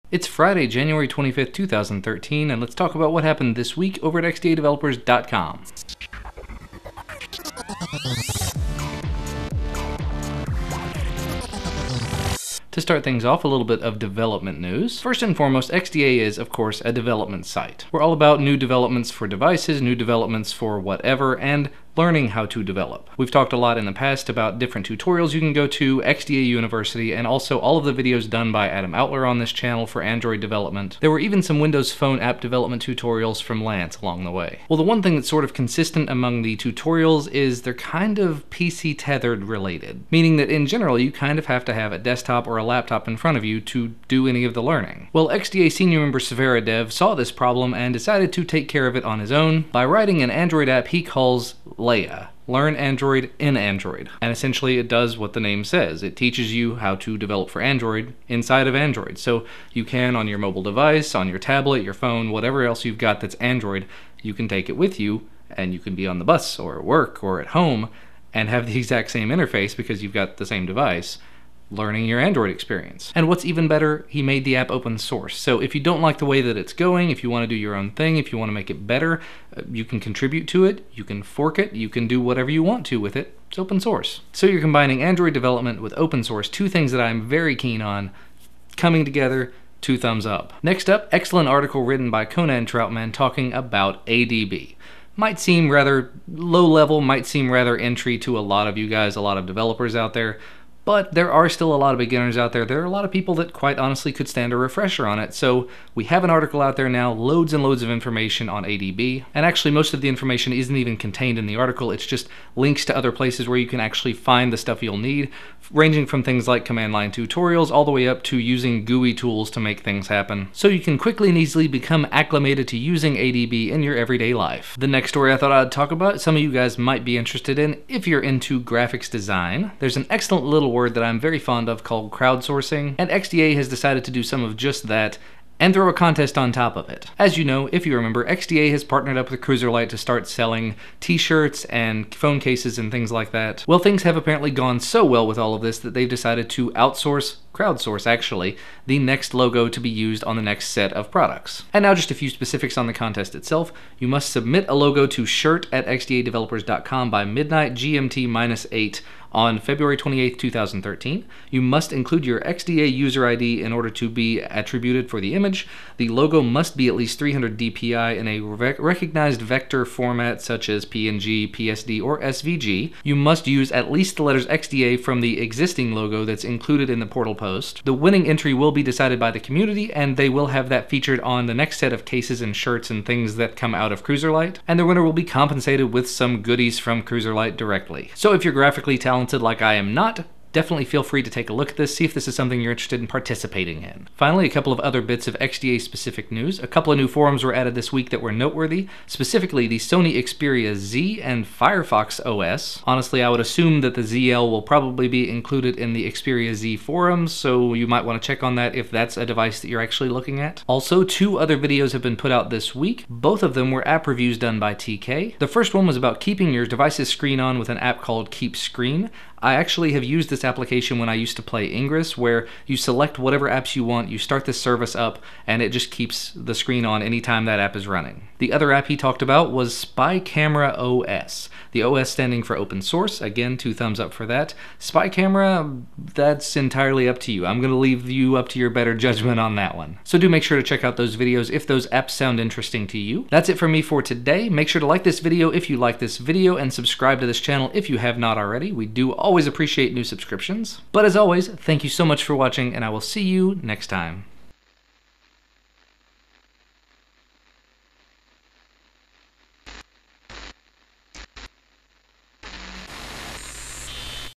Learning Android Development inside Android, ADB Tutorials and an XDA Merchandise Contest!
A lot of great news stories hit the news portal at XDA-Developers.com this week. Jordan reviews all the important stories from this week. Jordan talks about learning Android development in Android. Jordan talks about the other videos released this week on XDA Developer TV. XDA Developer TV Producer TK release two app reviews this week, one for KeepScreen and one for Spy Camera OS.

Additionally, this week Jordan talks about learning ADB and the contest for the next XDA merchandise. Finally, Jordan talks about XDA adding forums for the Sony Xperia Z and Firefox OS.  Pull up a chair and check out this video.

Links to stories mentioned:

App Review: Select Apps to Keep Your Screen On with KeepScreen -- XDA Developer TV
App Review: Sneaky Pictures Now with Spy Camera OS -- XDA Developer TV